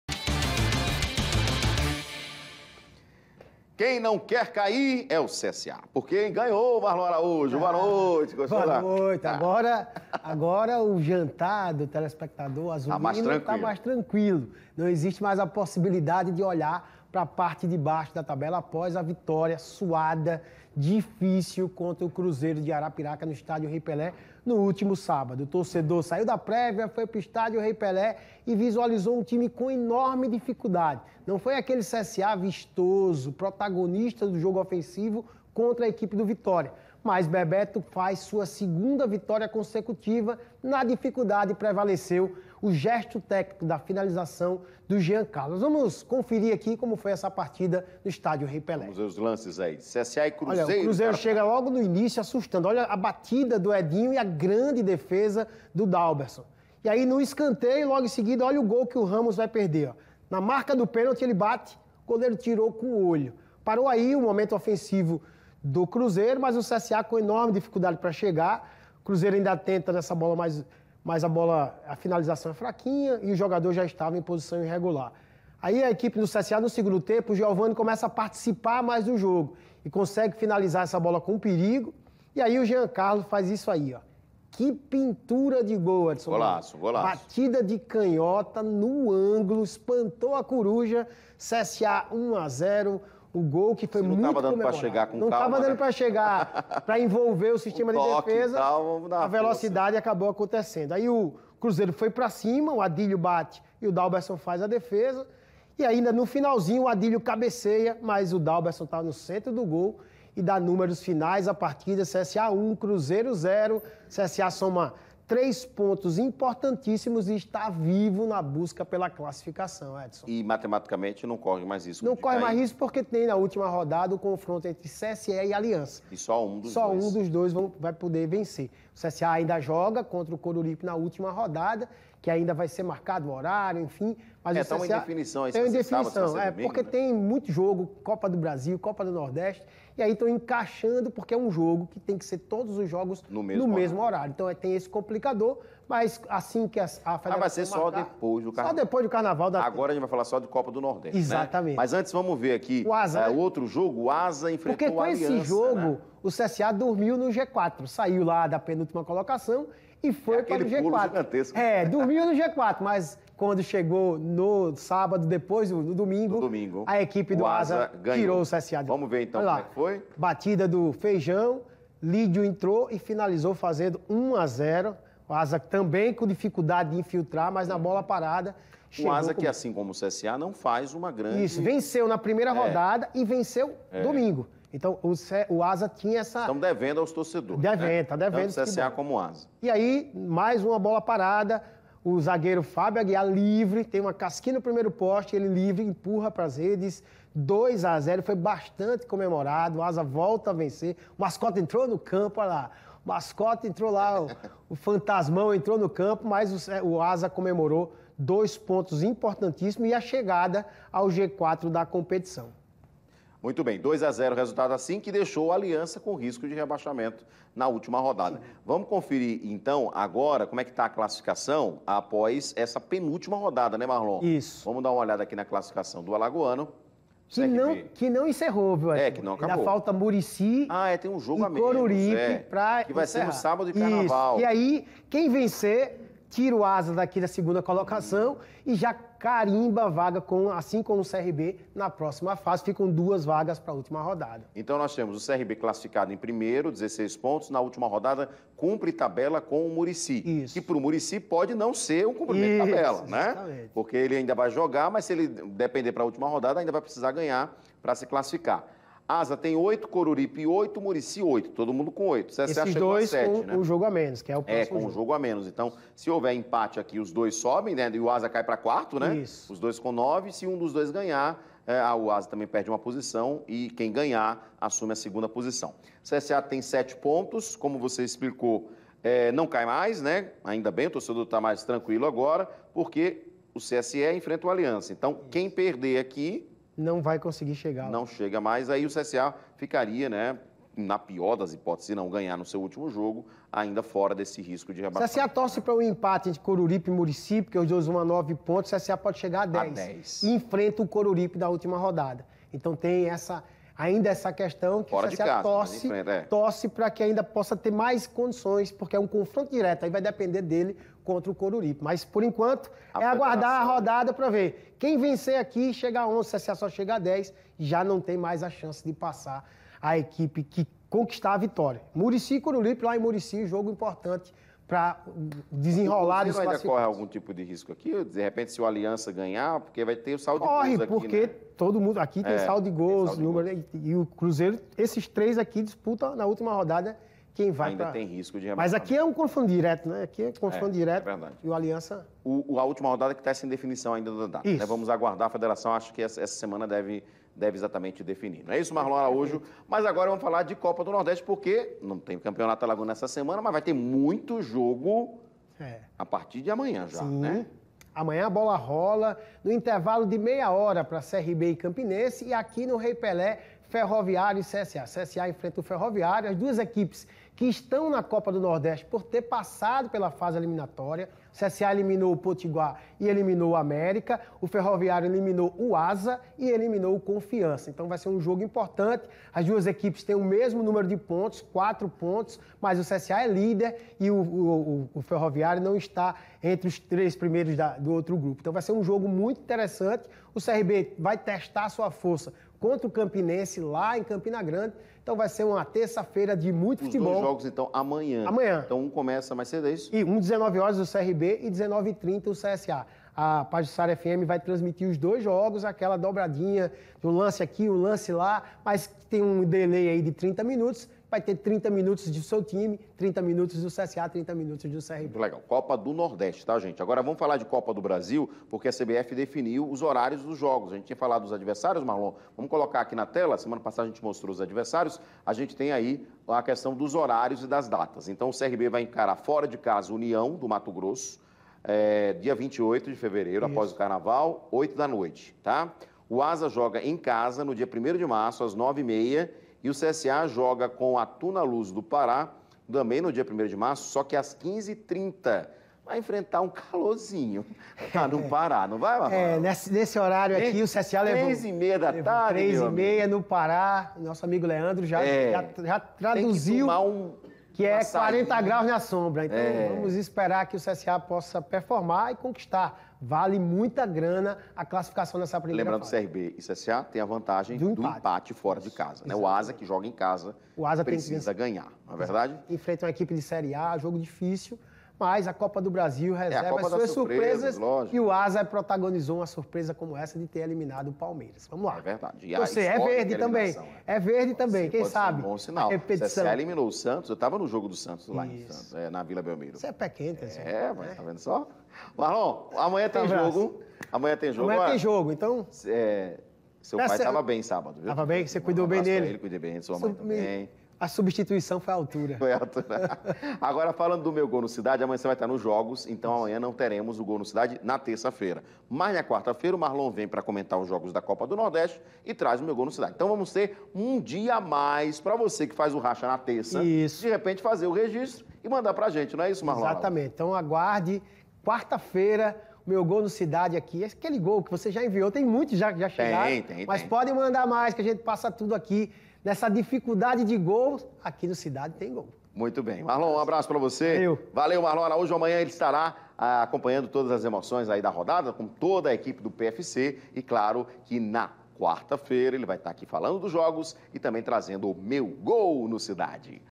Futebol: Campeonato Alagoano de Futebol 2023
No finalzinho, CSA vence o Cruzeiro e ainda briga pelas semifinais do alagoano.